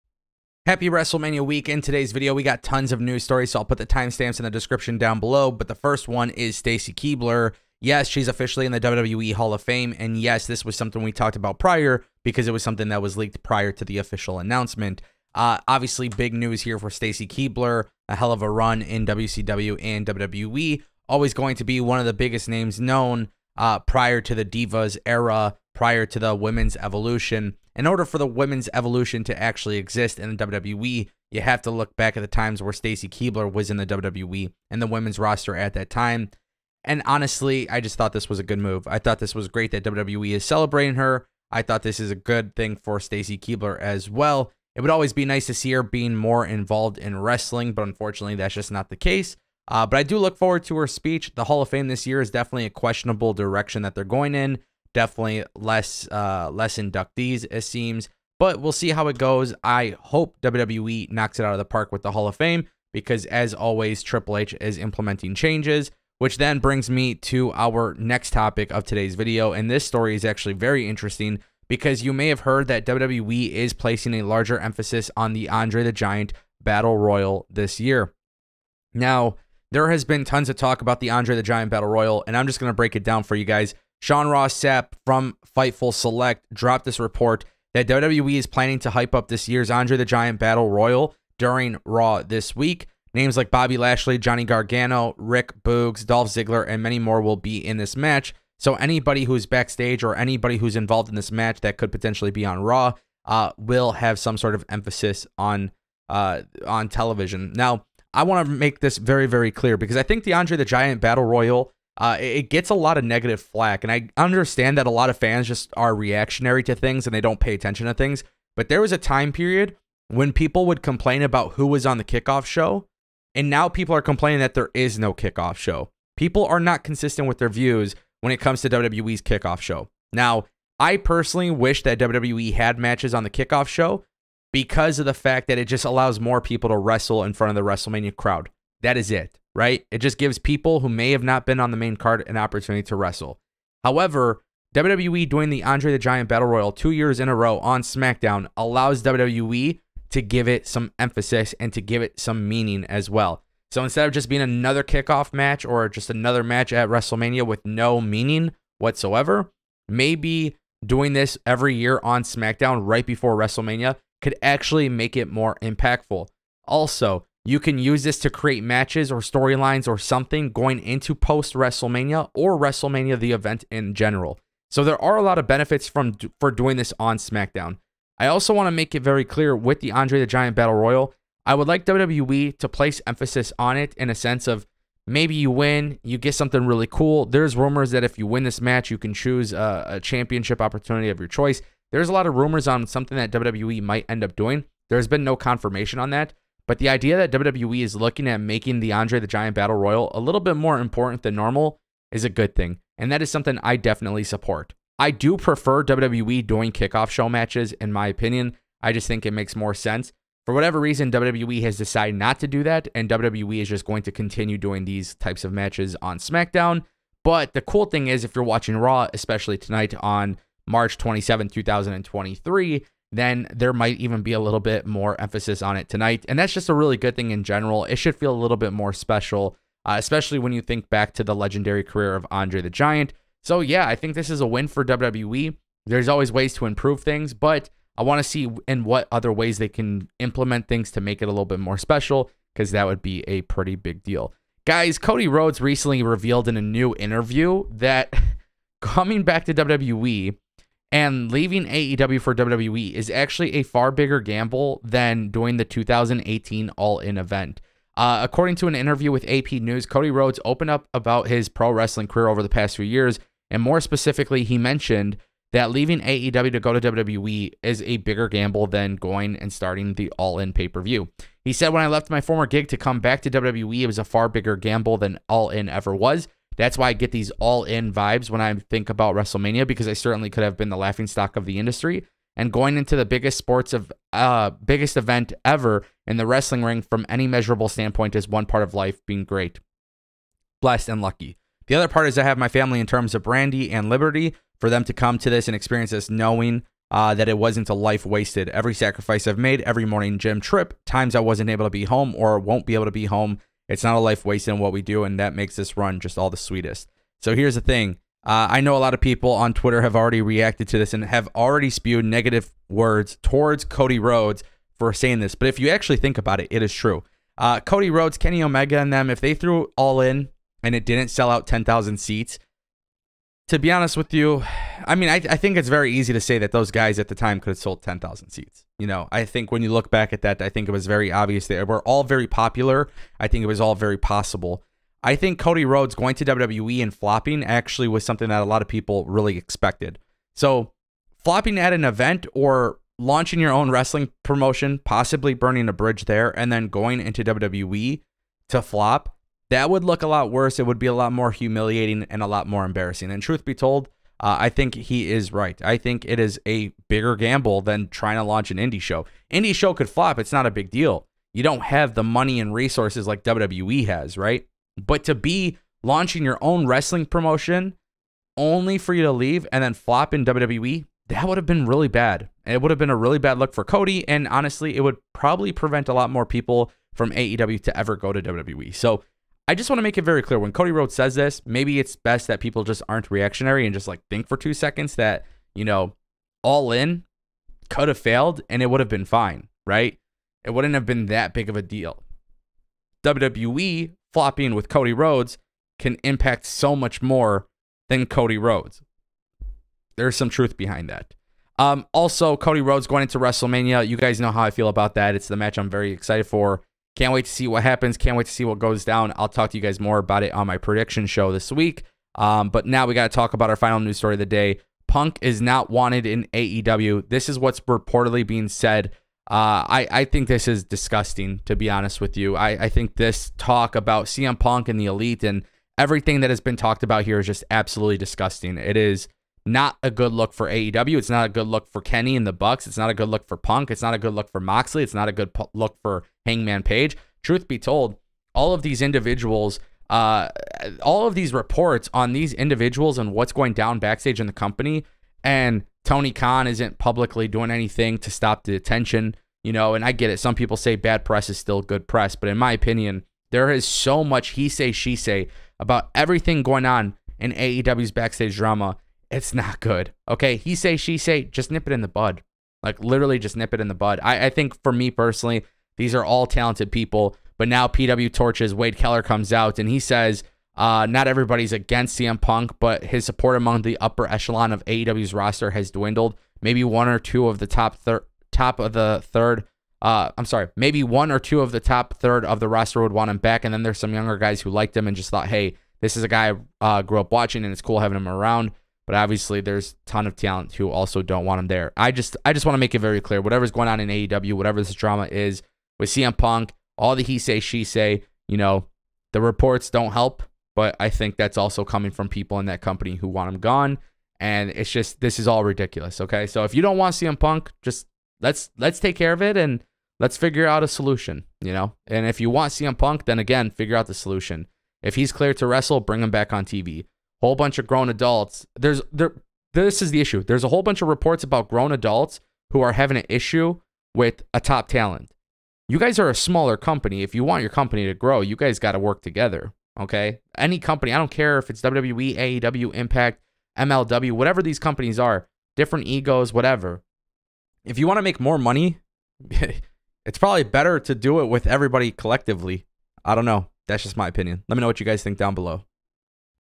AEW Doesn't Want CM Punk.. Cody Rhodes Speaks Out! WWE Changes Revealed & More Wrestling News!
Wrestling News topics
0:00 NEW WWE Hall of Fame Inductee Revealed
1:11 WWE Making Changes to Andre The Giant Memorial Battle Royal
4:04 Cody Rhodes Speaks Out! CODY RHODES SAYS LEAVING AEW WAS A ‘FAR BIGGER GAMBLE’ THAN 2018 ALL IN EVENT
6:52 AEW Wrestlers Want CM Punk Out Of Company